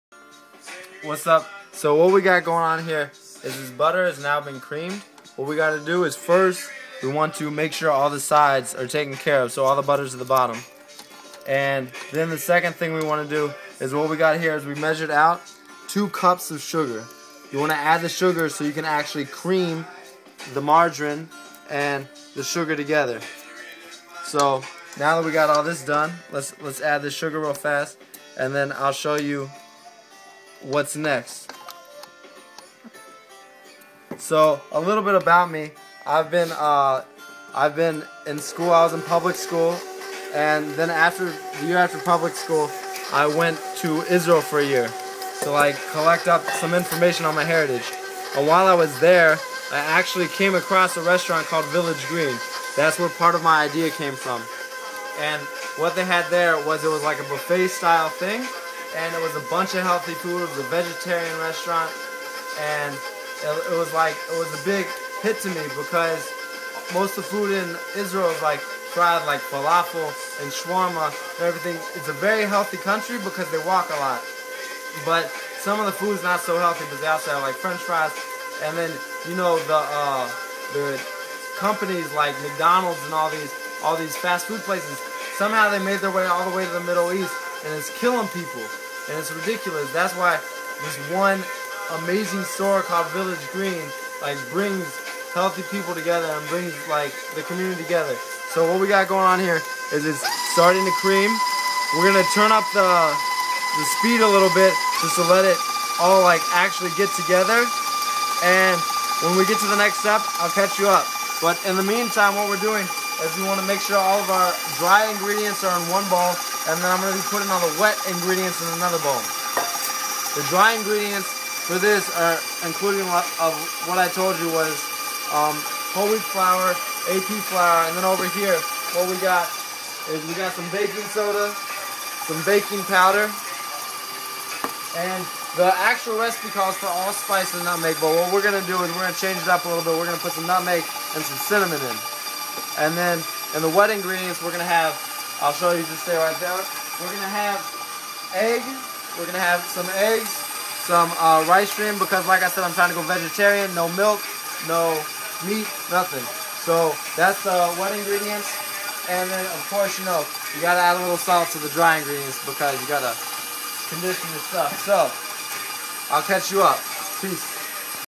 The Village Green Experience - Chocolate Chip Banana Muffins Part 2
Second portion of my first segment. I continue the demo and talk more about my inspirations regards "The Village Green Experience."